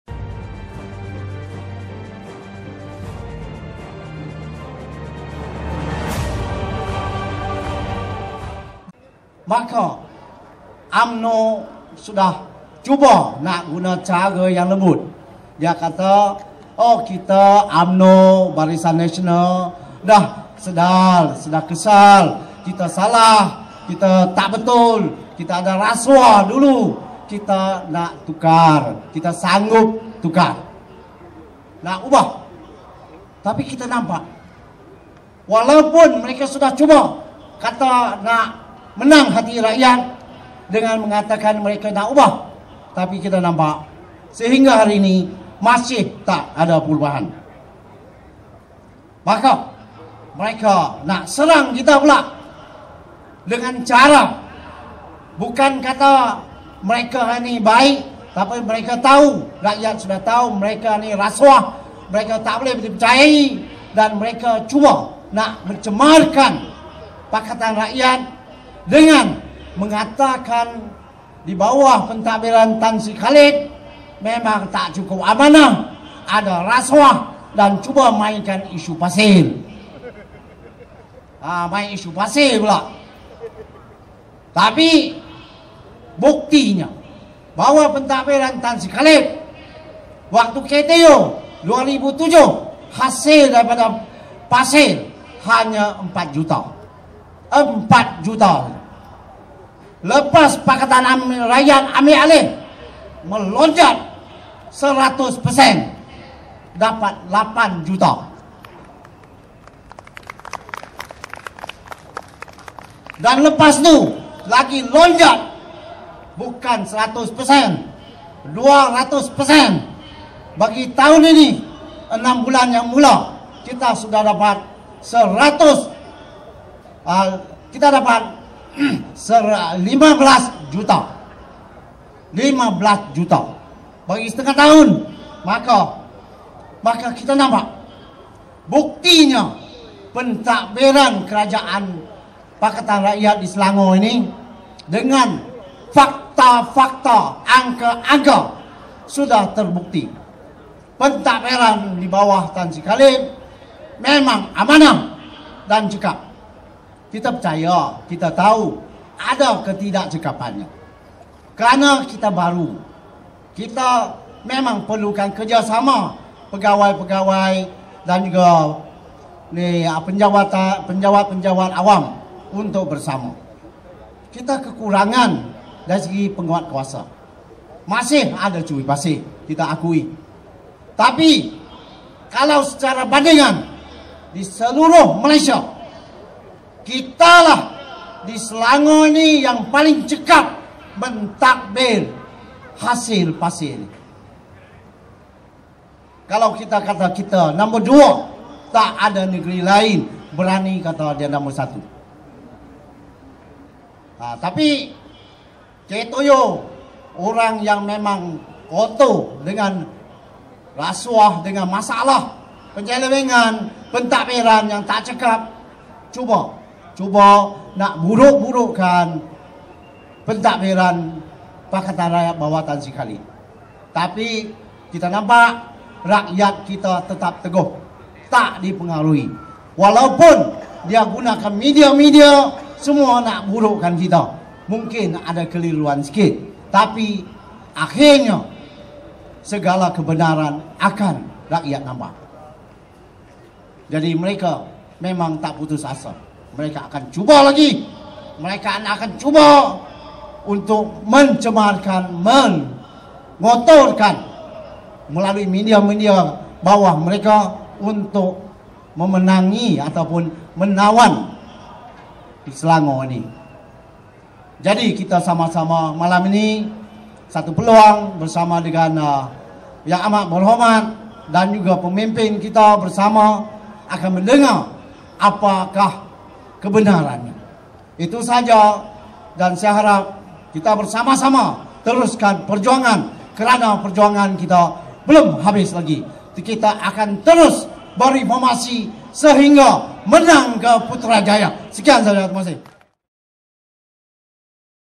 (Kajang Part 2) Lee Kim Sin (Cikgu Lee): People Distrust UMNO Barisan Nasional
Kajang Stadium, Kajang, Selangor 06/08/2010